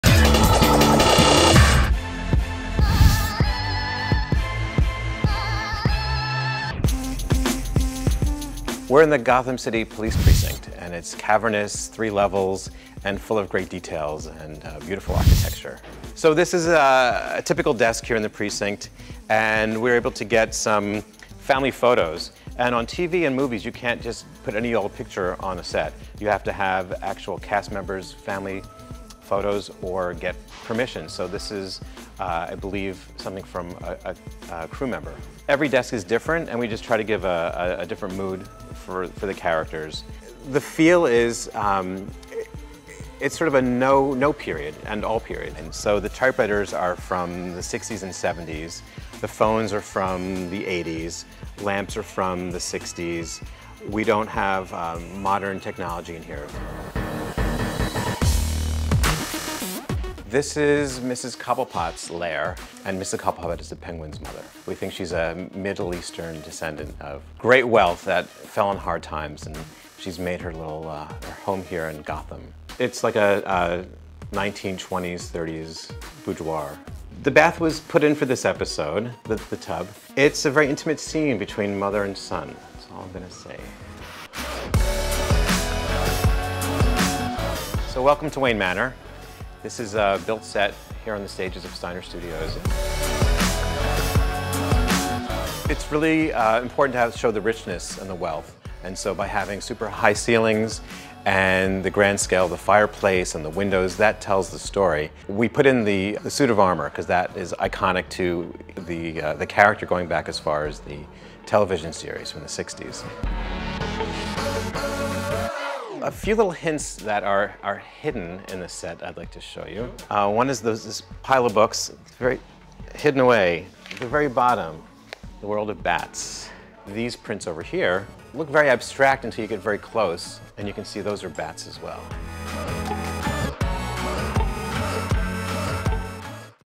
Gotham: Take a tour of the set!
Gotham set decorator Andrew Baseman gives us a guided tour of the GCPD precinct, Wayne Manor and more.